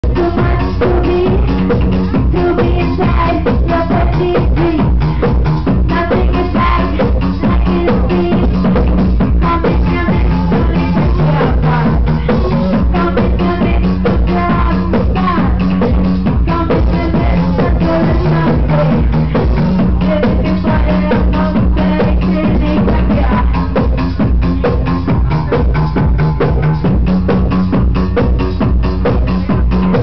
vj azz +blanali +wolfgang gewalt,club optimal@ flex, vienna , wien 維也納 電音跳舞文化 2006
8th aug with crew optimal, dj marvin matula, flowkee, slick, blanali feat wolfgang gewalt, live visuals by vj azz